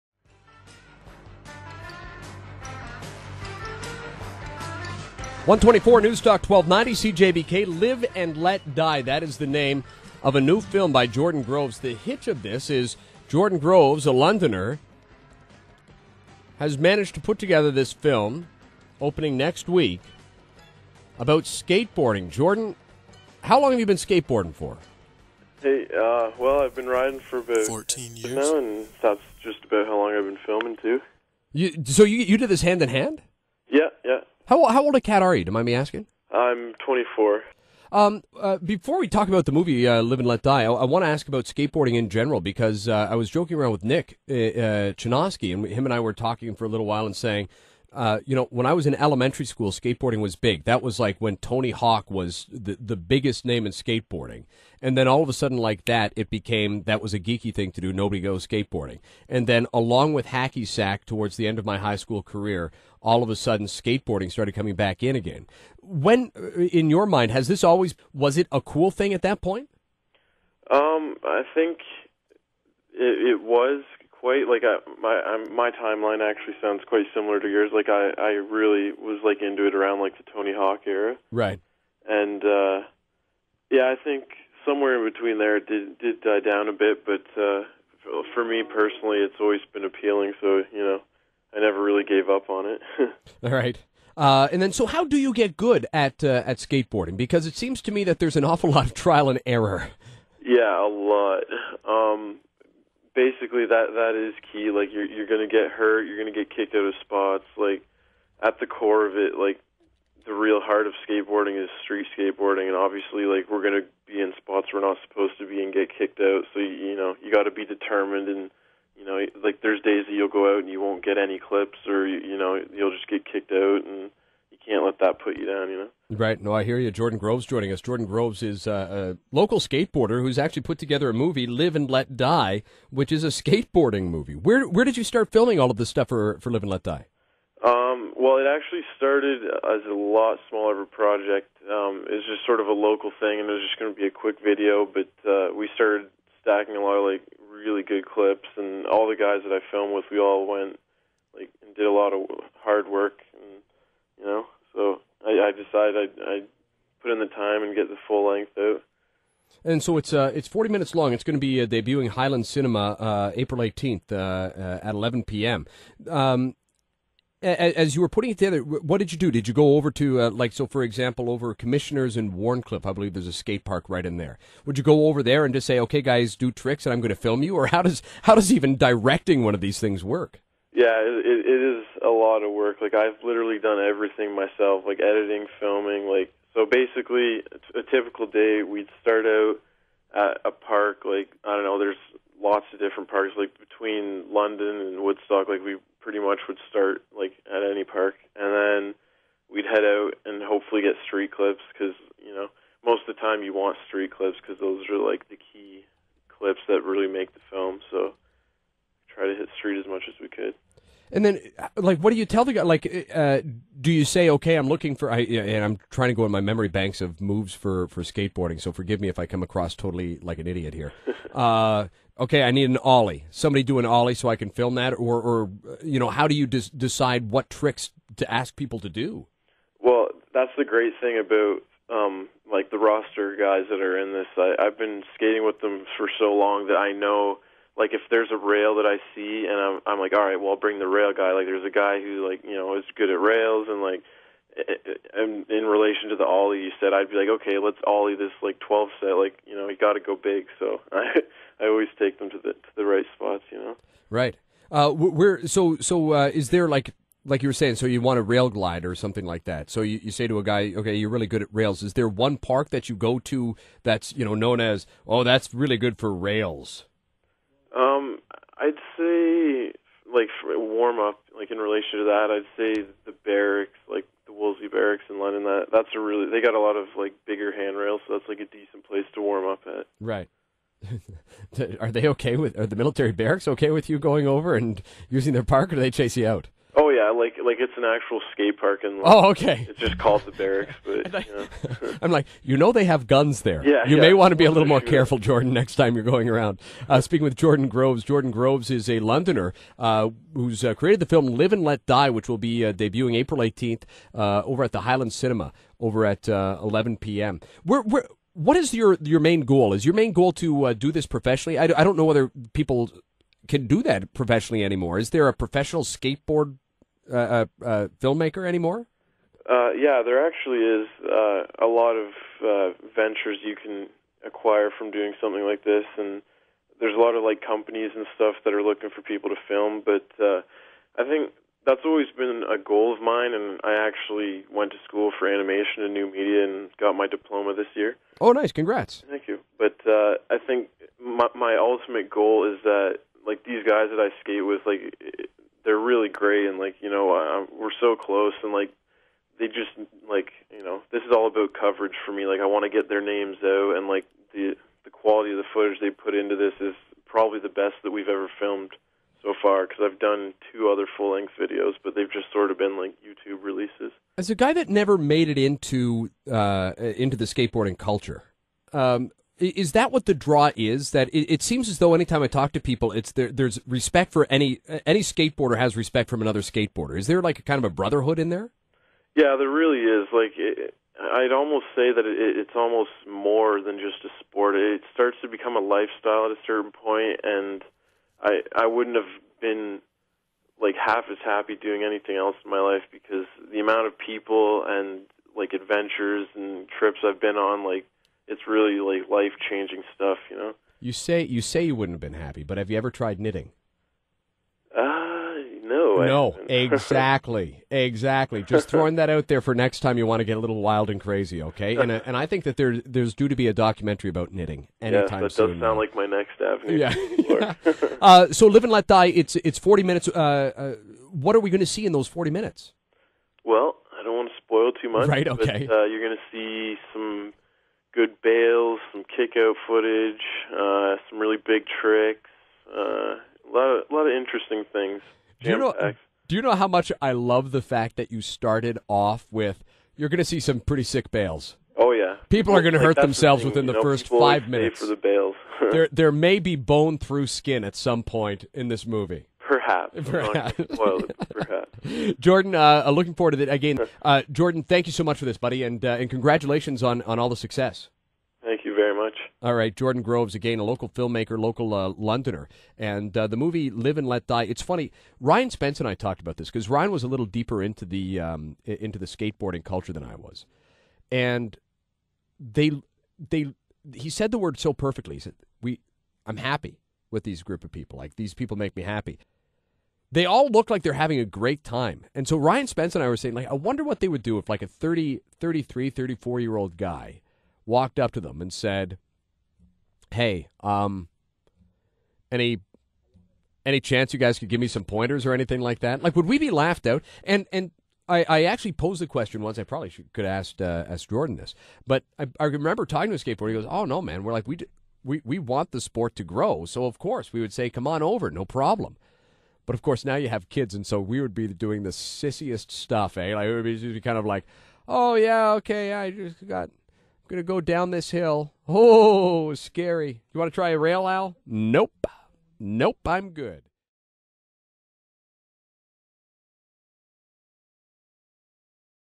Jordan Groves "Live and Let Die" CJBK Radio Interview
Alan Coombs; Host of London at Large, Newstalk 1290 CJBK, Interviewed me about my upcoming full length skateboard video. Despite working a midnight shift the night before ( you can actually hear me wake up as the interview goes on) , it was a great interview with lots of useful information on my past as well as the video.

The premier date is April 18, 2014, at the Hyland Cinema in London, Ontario @ 11pm (10 dollars cash only, prizes a plenty)